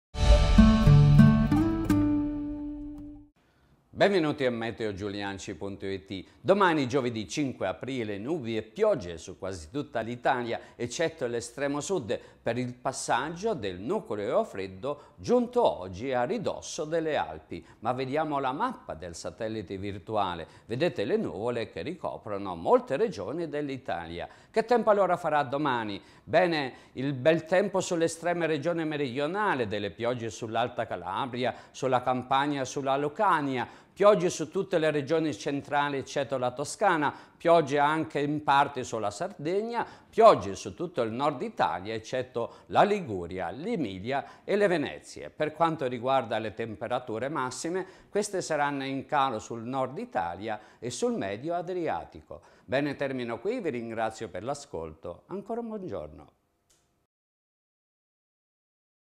Meteo di domani, giovedì 5 aprile 2012.
Le previsioni del Colonnello Mario Giuliacci per domani, giovedì 5 aprile 2012.